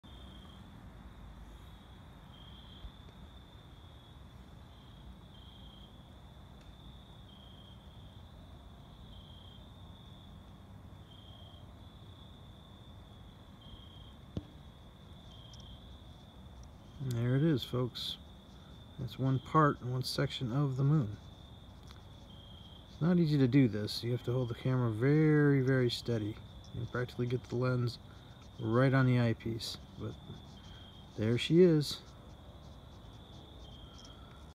Video image of the moon 🌕 through the lens of my telescope 🔭.
Brief description by me of my telescope and what I’m doing. That’s all. Hope you enjoy.